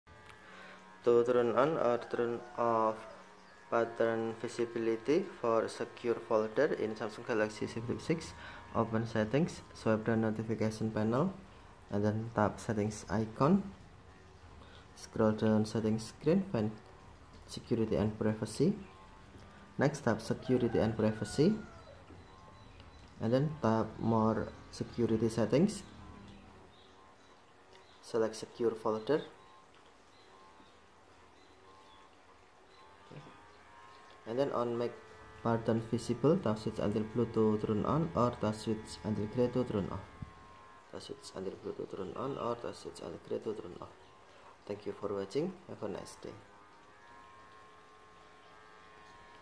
Samsung Galaxy Z Flip 6: How to Turn On/Off Pattern Visibility for Secure Folder (Android 14)
The **Secure Folder** on the Samsung Galaxy Z Flip 6 is designed to protect your sensitive apps and files from unauthorized access. One of the features you can manage is **Pattern Visibility**. This feature determines whether your unlock pattern is visible on the screen when you're entering it, adding a layer of discretion to your security. If you want to turn pattern visibility on or off for your Secure Folder on Android 14, this guide will walk you through the steps.

**Steps to Turn On Pattern Visibility for Secure Folder:**

1. **Open Settings:**
   - Unlock your Samsung Galaxy Z Flip 6.
   - Swipe down from the top of the screen to access the Quick Settings panel.
   - Tap the gear icon to open **Settings**.

2. **Go to Biometrics and Security:**
   - Scroll down and select **Biometrics and Security** from the options.

3. **Access Secure Folder Settings:**
   - Under the **Security** section, tap on **Secure Folder**.
   - Enter your current lock method (PIN, password, or pattern) to access the Secure Folder settings.

4. **Enable Pattern Visibility:**
   - Inside the Secure Folder settings, tap on the three dots in the top-right corner and select **Settings**.
   - Look for the option labeled **Pattern Visibility**.
   - Toggle the switch **On** to make your unlock pattern visible while entering it.

5. **Test the Visibility:**
   - Lock your Secure Folder and then attempt to unlock it using the pattern to see if it is visible on the screen.

**Steps to Turn Off Pattern Visibility for Secure Folder:**

1. **Repeat Steps 1 to 3:**
   - Open **Settings**, navigate to **Biometrics and Security**, and access the Secure Folder settings.

2. **Disable Pattern Visibility:**
   - In the **Pattern Visibility** option, toggle the switch **Off**.

3. **Test the Settings:**
   - After disabling, lock your Secure Folder again and attempt to unlock it to ensure the pattern is no longer visible.

**Why Turn On Pattern Visibility?**

- **Ease of Use:** If you're in a well-lit environment or are confident that no one is watching, having the pattern visible can make it easier to enter quickly.
- **Quick Recognition:** It may help you enter the pattern more accurately if you have trouble recalling it.

**Why Turn Off Pattern Visibility?**

- **Enhanced Privacy:** Keeping your pattern hidden adds an extra layer of security, preventing anyone from watching you enter your unlock method.
- **Discretion:** If you're in a public setting, it’s less likely that someone will see and memorize your unlock pattern.

**Conclusion:**
Managing **Pattern Visibility** for the Secure Folder on your Samsung Galaxy Z Flip 6 allows you to customize your security settings based on your comfort level and environment. Whether you prefer to keep your pattern visible for convenience or hidden for privacy, following the steps above will help you easily toggle this feature on or off in Android 14.